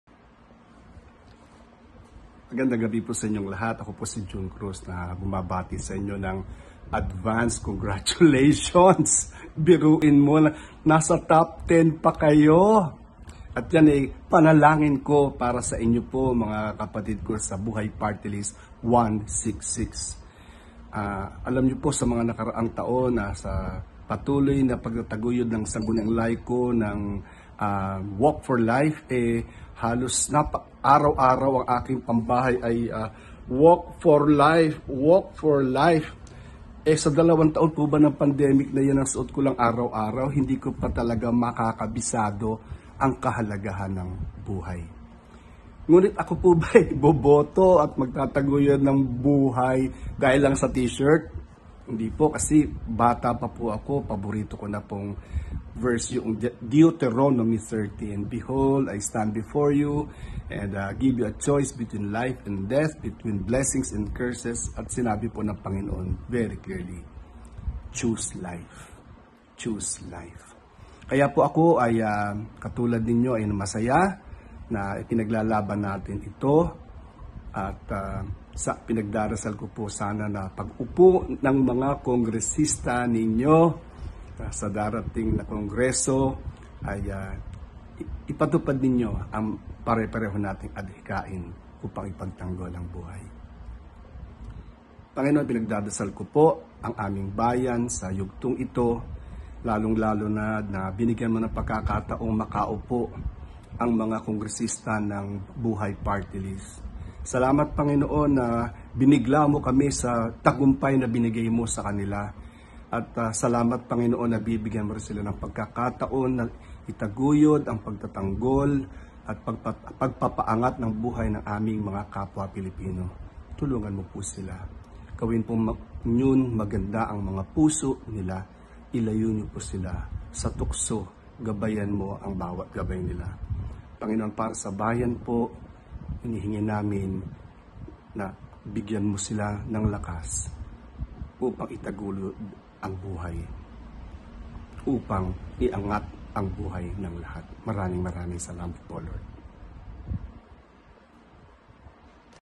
Bro. Jun Cruz of Sangguniang Laiko ng Pilipinas for Dasal Para sa Bayan of BUHAY 166
Online Grand Prayer Rally and Praisefest / Miting de Avance
Thank you for your support of BUHAY 166!